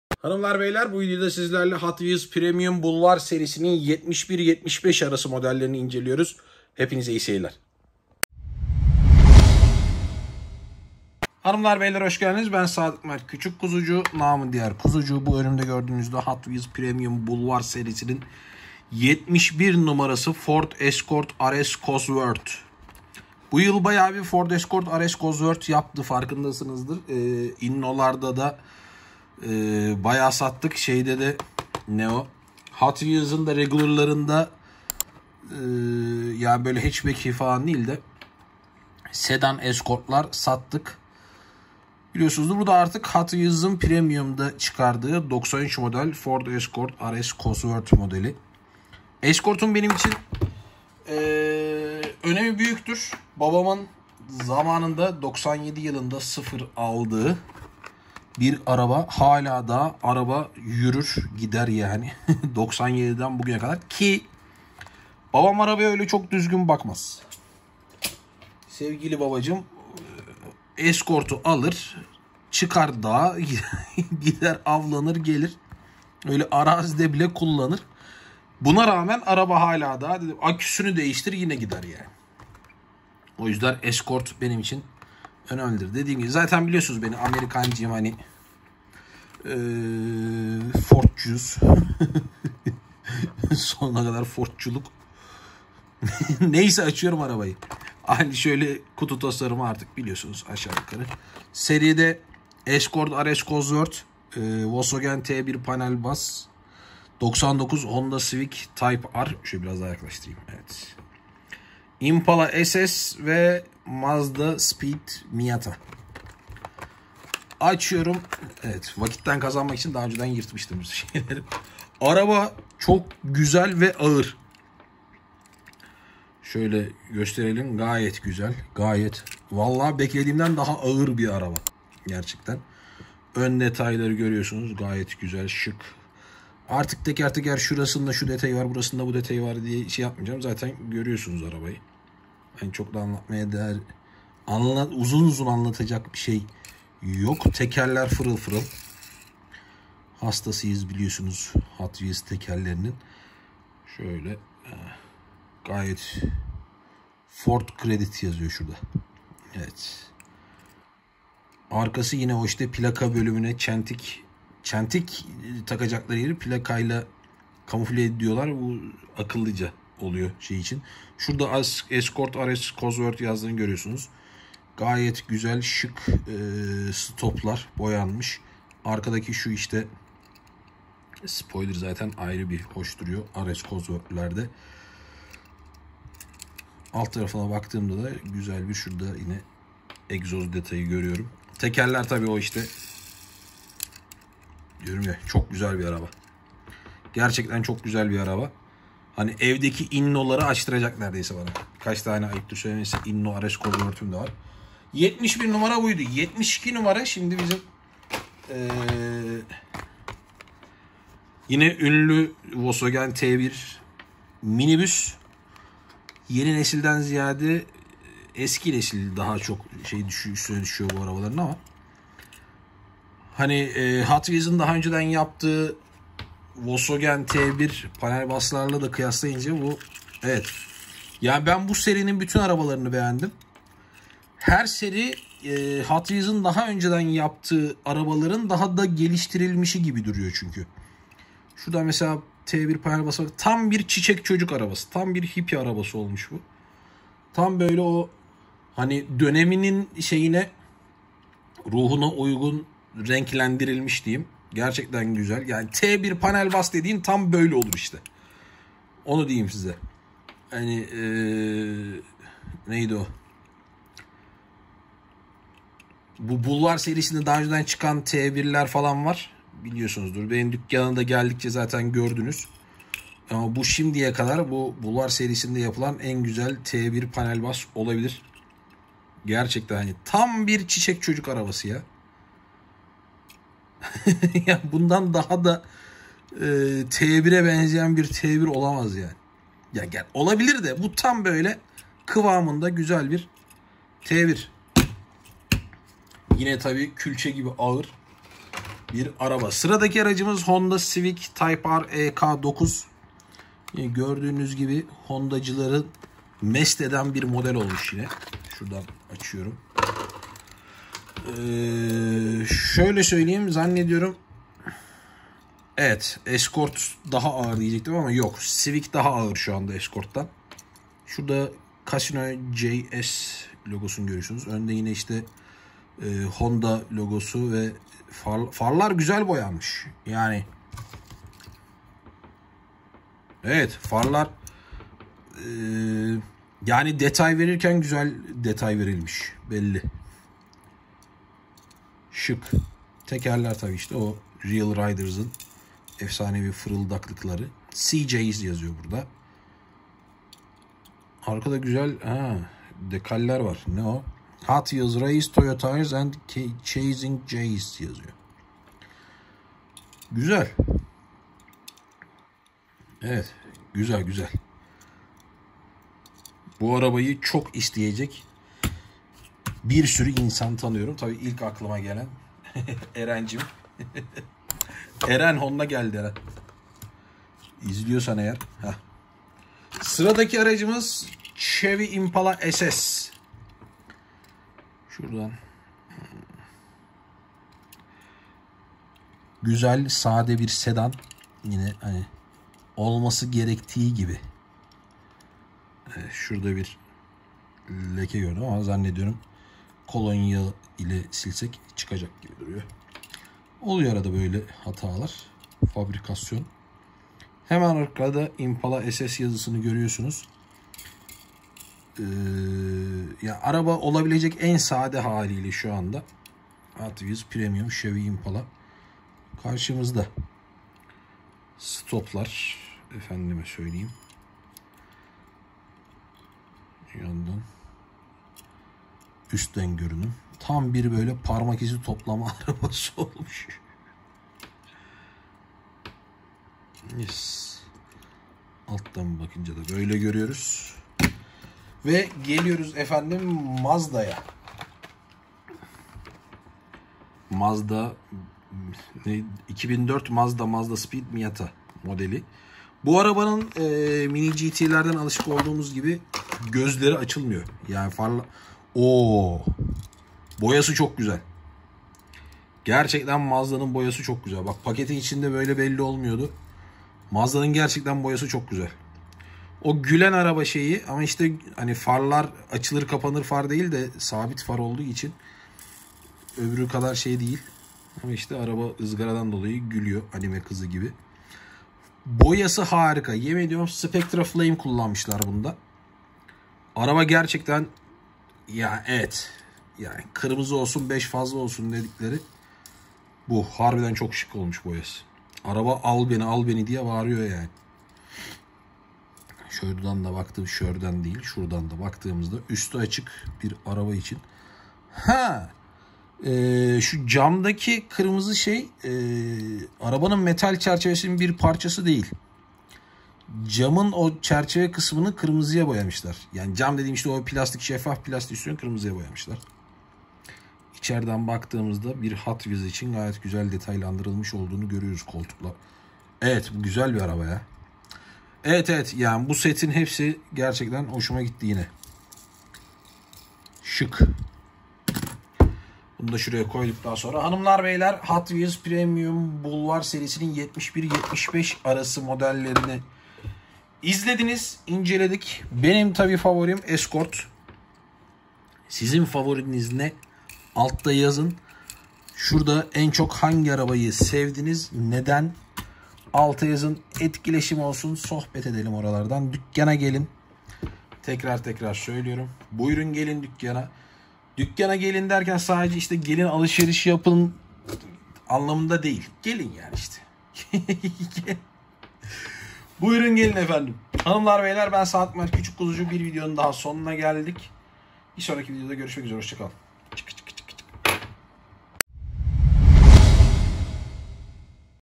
Hot Wheels Premium Boulevard 71 - 75 İNCELEME | Ford - Honda - Volkswagen - Mazda - Chevy
Hot Wheels Premium Boulevard 71 - 75 modelleri sizlerle efe'm :)